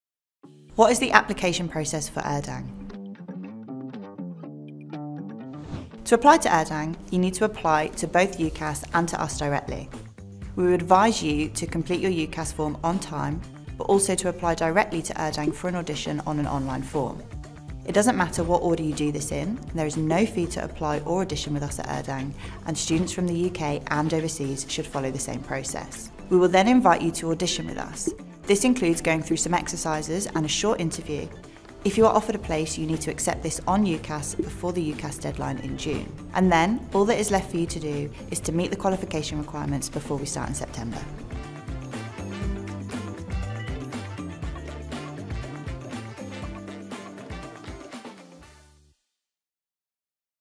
Urdang, City, University of London: What is the application process for Urdang?
For more information about studying at the Department of Performing Arts